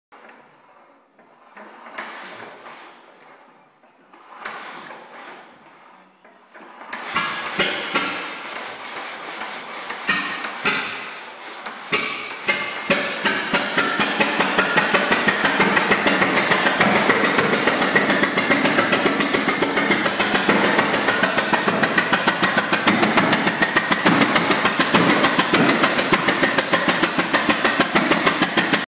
1919 IHC Titan 10-20
1919 IHC Titan 10-20 after routine maintenance
Hand started 11/02/2012